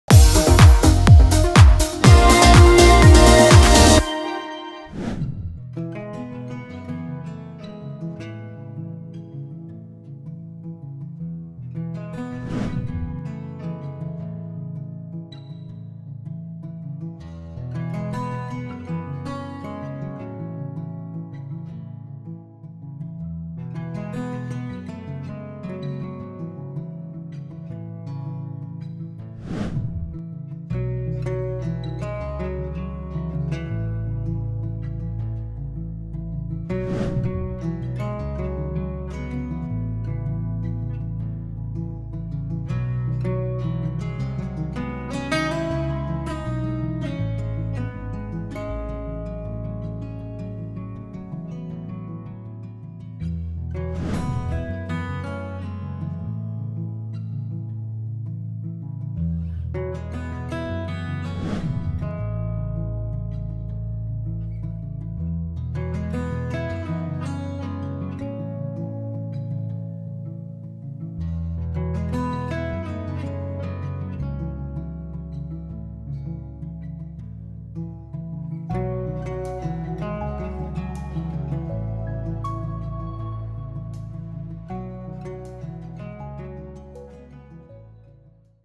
Venna Melinda dan Ferry Irawan Nikah Maret 2022 di Bali
KAPANLAGI.COM - Venna Melinda dan Ferry Irawan sepakat akan mengundang mantan ke pernikahan mereka yang akan digelar dalam beberapa bulan mendatang. Apa alasan memilih Bali untuk menikah? Simak penuturan selengkapnya di video berikut ini.